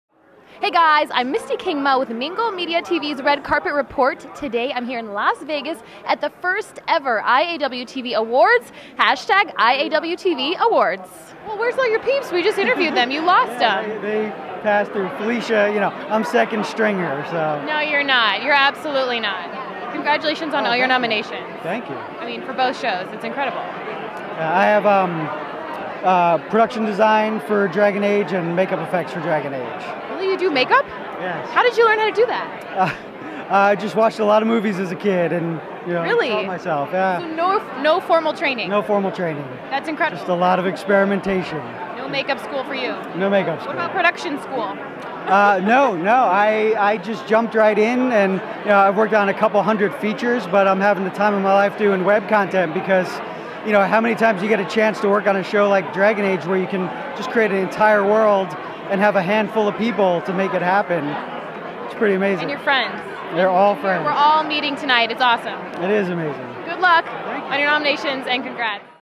Greg Aronowitz at IAWTV Awards Red Carpet: CES 2012 Las Vegas
Mingle Media TV and host Misty Kingma were in Vegas at CES 2012 and covered the inaugural IAWTV Awards. The International Academy of Web Television (IAWTV) Awards is the first award show within the Web television industry to be presented by content creators, for content creators. From studio content producers to YouTube celebrities, those successfully creating entertainment for digital platforms were celebrated via 33 categories honoring web series and online talent, in front of the camera and behind the scenes. For more information about the IAWTV Awards visit their For more coverage of this event and others, watch our video interviews from the Red Carpet and follow us on Twitter and Facebook